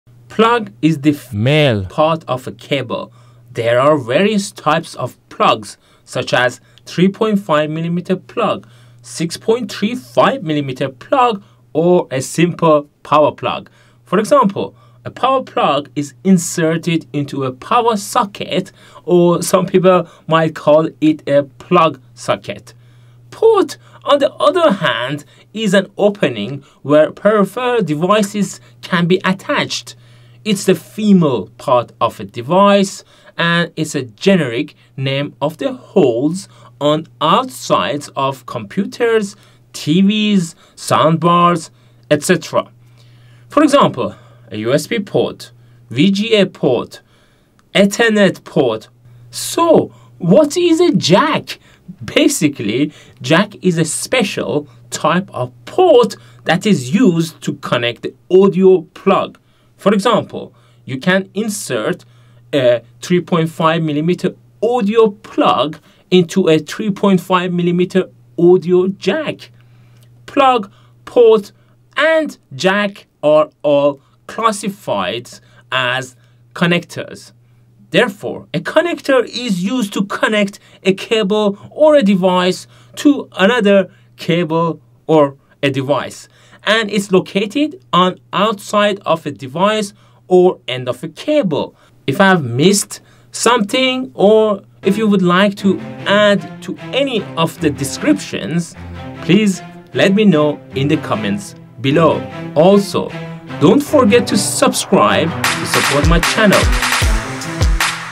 Difference between Plug, Port, Jack and Connector
Do you want to know what is the difference between Plug, Port, Jack and Connector! Plug is the male part of a cable. There are various types of plugs, such as 3.5 mm plug, 6.35 mm plug or a simple power plug. For example a power plug is inserted into a power socket or some people might call it a plug socket. Port on the other hand is an opening where peripheral devices can be attached.  It's the female part of a device and it’s a generic name of the holes on outsides of computers, TVs, soundbars, etc. For example, a USB port, VGA port, Ethernet Port. So, what is a Jack? Basically, Jack is a special type of port that is used to connect the audio plug. For example, you can insert a 3.5 mm audio plug into a 3.5 mm audio jack. Plug, Port, and Jack are all classified as connectors. Therefore, a connector is used to connect a cable or a device to another cable or device and it's located on outside of a device or end of a cable.